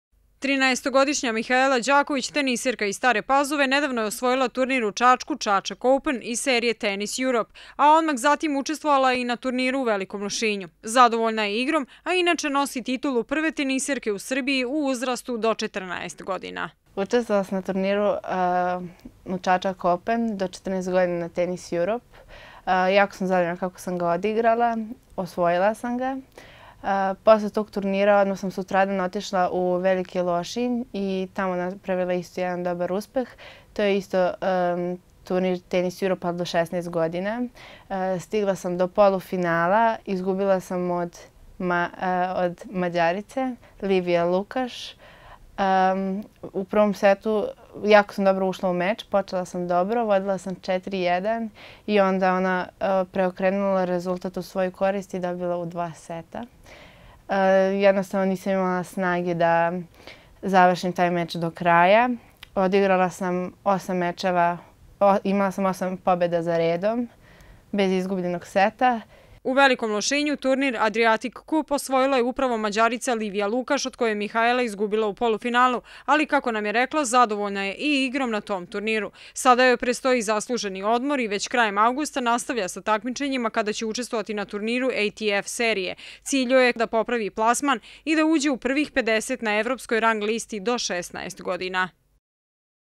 Mihaela Đaković
Staropazovčanka Mihaela Đaković, prva teniserka u Srbiji u uzrastu do 14 godina, nedavno je osvojila turnir u Čačku.